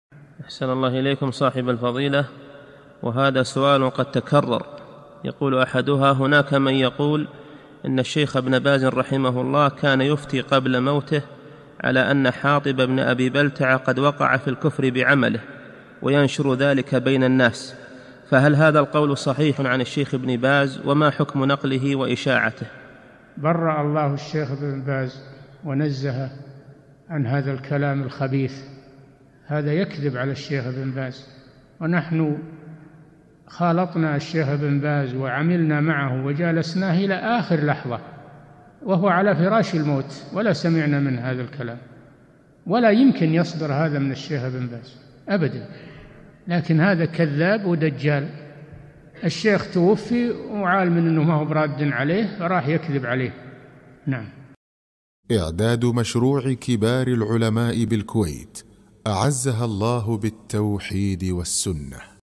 [410 -560] هل نقل عن الشيخ ابن باز من أن حاطب بن أبي بلتعة قد وقع في الكفر بعمله؟
-"السؤال: وهذا سؤال قد تكرر، يقول أحدها: هناك من يقول: إن الشيخ ابن باز رحمه الله كان يفتي قبل موته على أن حاطب بن أبي بلتعة قد وقع في الكفر بعمله، وينشر ذلك بين الناس، فهل هذا القول صحيح عن الشيخ ابن باز وما حكم نقله وإشاعته؟
الجواب: برَّأ الله الشيخ ابن باز ونزهه عن هذا الكلام الخبيث، هذا يكذب على الشيخ ابن باز ونحن خالطنا الشيخ ابن باز وعملنا معه وجالسناه إلى آخر لحظة وهو على فراش الموت، وما سمعنا منه هذا الكلام، ولا يمكن يصدر هذا من الشيخ ابن باز أبدا، لكن هذا كذاب ودجال، الشيخ توفي ويعلم أنه ما هو برادٍ عليه فذهب يكذب عليه.
للاشتراك في قناة الشيخ صالح الفوزان
سؤال وجواب من شرح السنة للبربهاري ( 560 ) مقطع

للاستفسار والإقتراحات والدعم
رئيس الفريق
لمشروع كبار العلماء